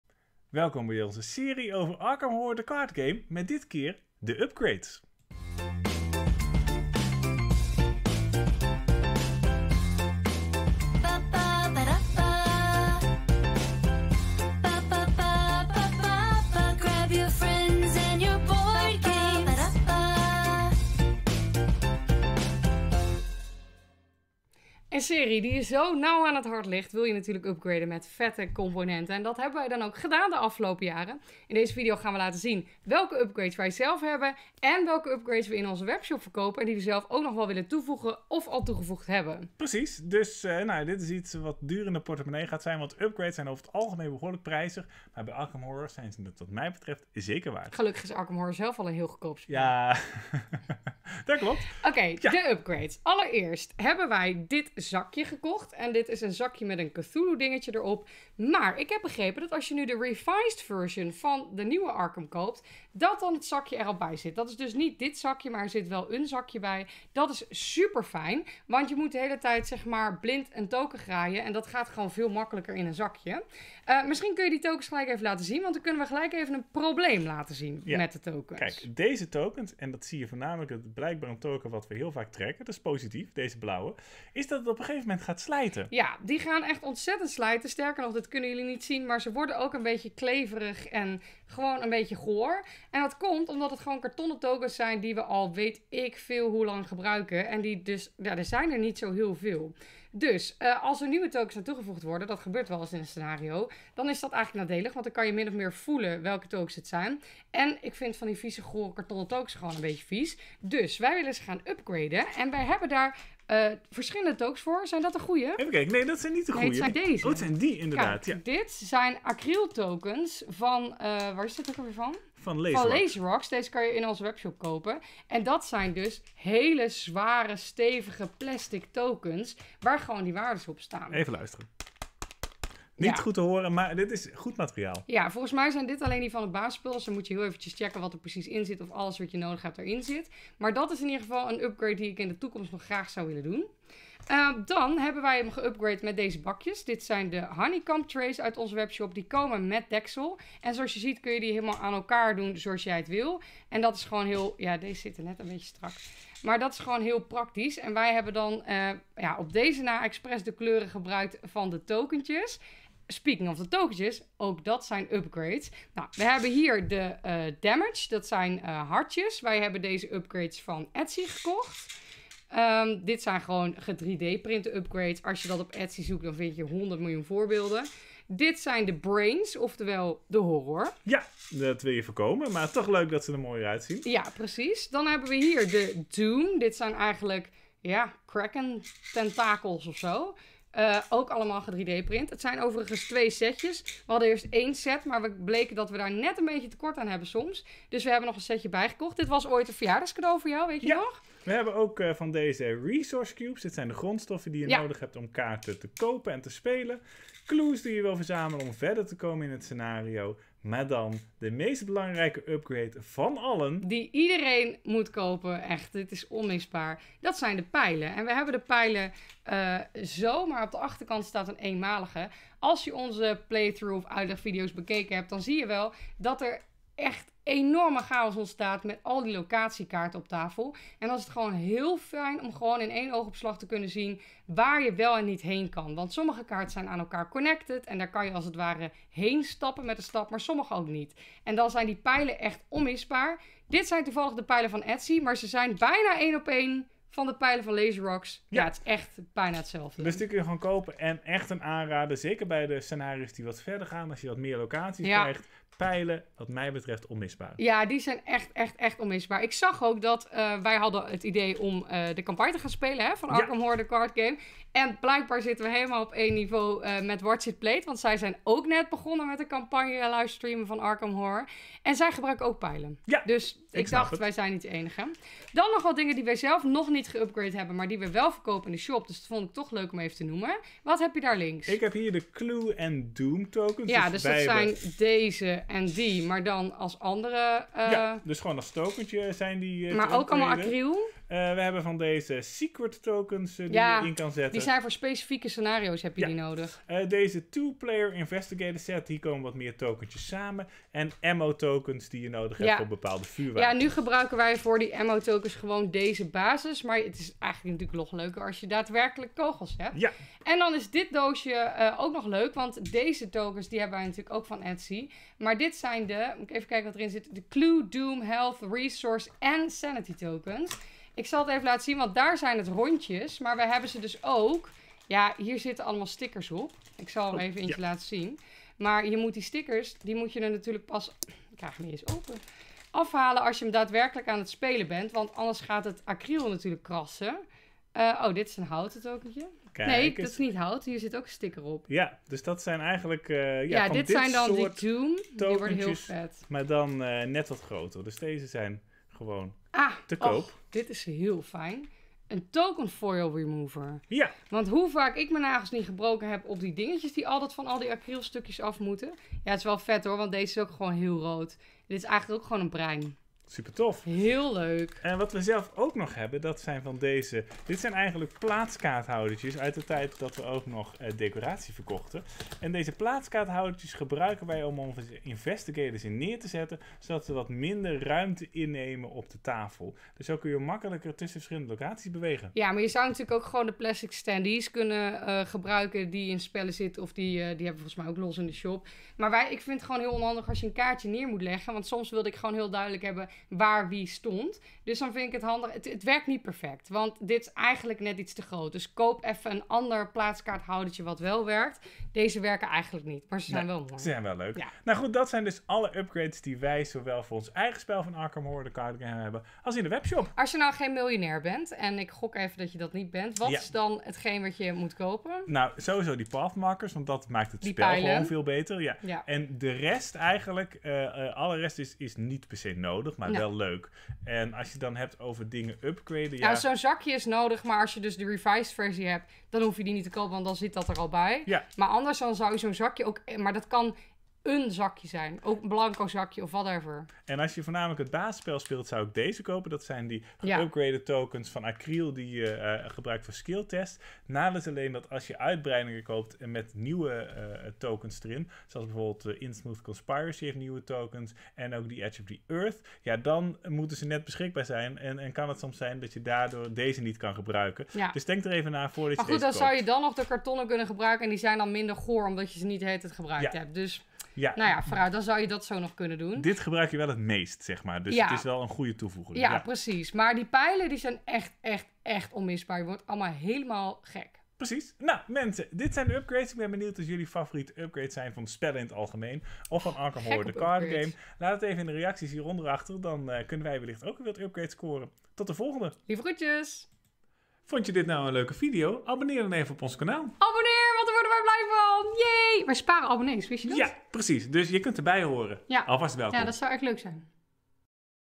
Arkham Horror The Card Game - Deze upgrades hebben wij! [Serie, deel 8]
We maken een serie over Arkham Horror The Card Game, één van onze lievelingsspellen. ↓ Meer info + linkjes.

► SHOP

► VOLG ONS